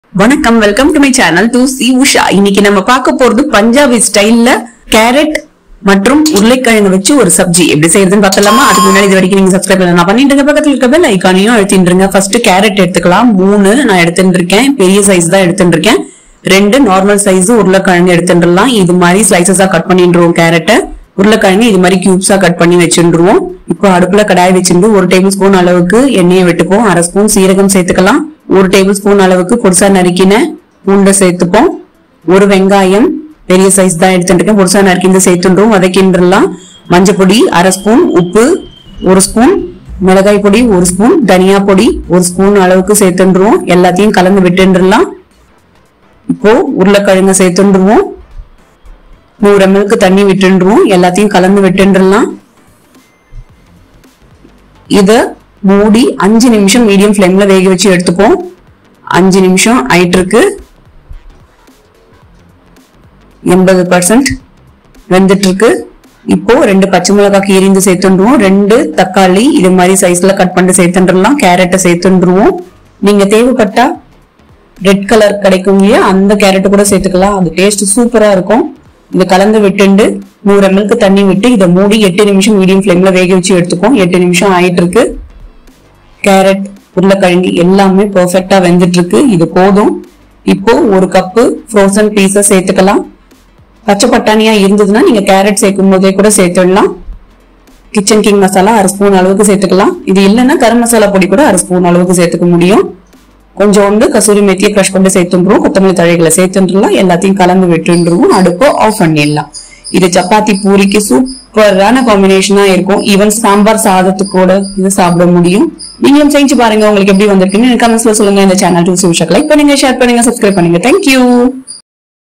பஞ்சாபி style உருளைக்கிழங்கு -கேரட் சப்ஜி|Aloo Gajar Matar Sabji|Potato Carrot Sabji
Urulaikizhangu Carrot Sabji:

Carrot(Big) – 3
Potato – 2

Finely chopped Garlic pods -1 tbsp
Onion – 1

Green chillies(slit) – 2
Tomato – 2

Frozen peas - one cup
Kitchen king masala/Garam masala powder - 1/2 spoon
Few Crushed Kasuri methi leaves

Punjabi style potato carrot green peas curry,aloo gajar matar sabji,urulaikizhangu carrot pattani sabji,punjabi style sabji,aloo sabji,tocusha,to c usha